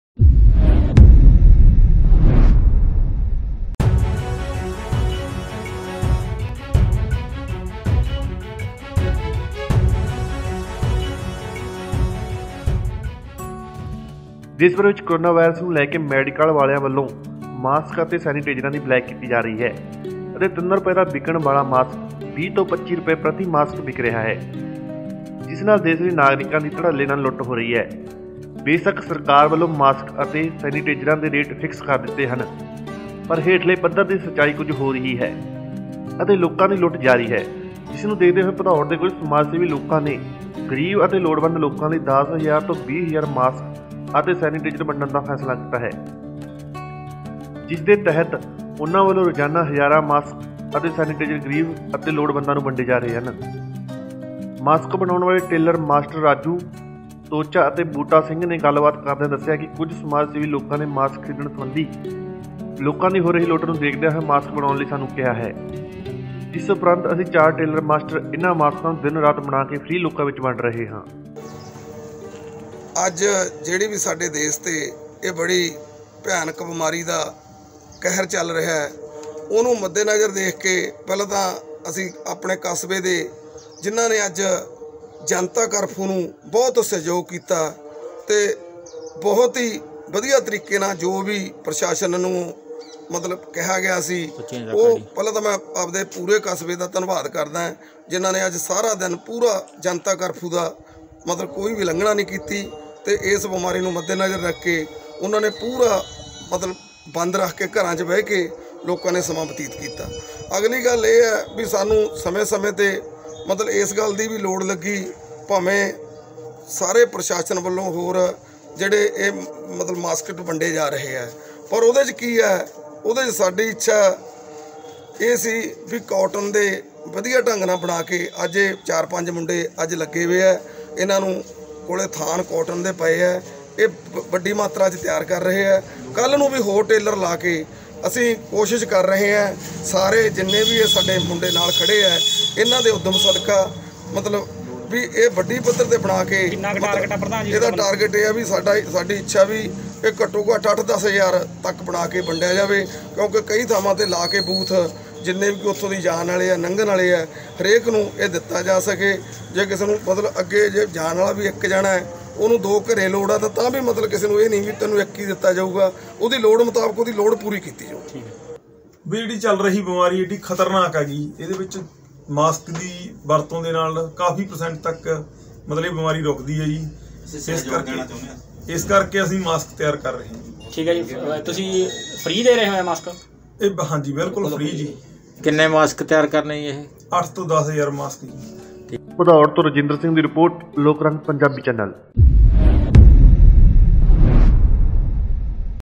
ਸਮਾਜ ਸੇਵੀ ਲੋਕਾਂ ਵੱਲੋਂ ਮਾਸਕ ਬਣਾ ਕੇ ਲੋੜਵੰਦਾਂ ਨੂੰ ਦਿੱਤੇ ਜਾ ਰਹੇ ਨੇ ਮੁਫਤ|ਮੈਡੀਕਲ ਵਾਲੇ ਮਚਾ ਰਹੇ ਨੇ ਲੁੱਟ
For Latest News Updates Follow Lok Rang Punjabi

Google+
Support